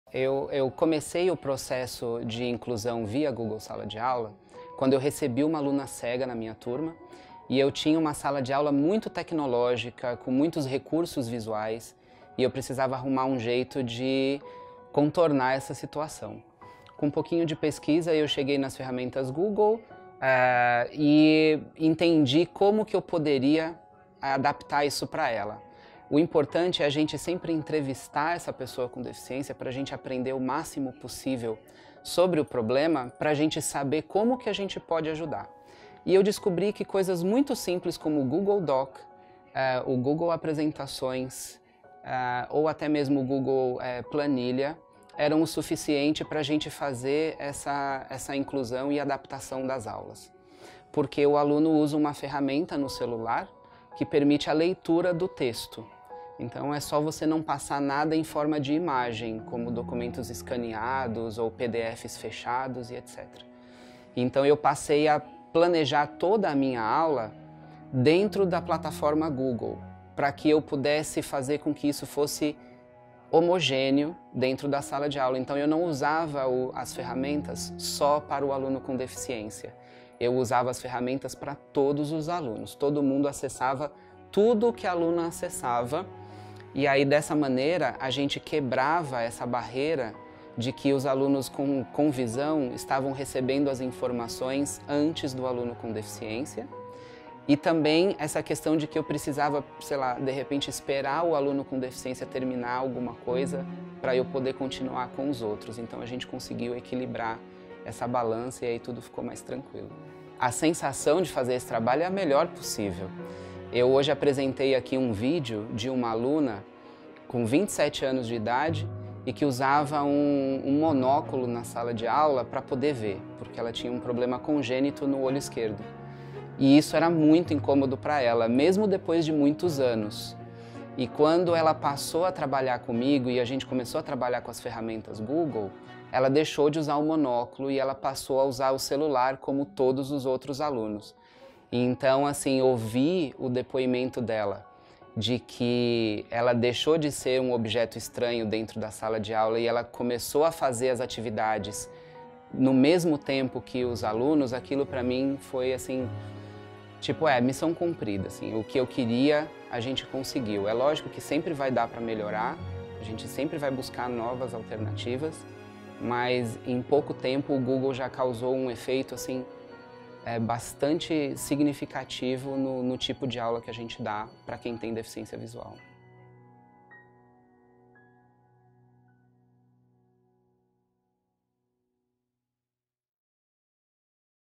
Acessibilidade. Tecnologia na educação para todos.
Case apresentado no Google durante o Encontro de Boas Práticas em Tecnologias Educacionais promovido pela Nuvem Mestra por Rodrigo Correia. Como a tecnologia na educação pode promover inclusão e acessibilidade.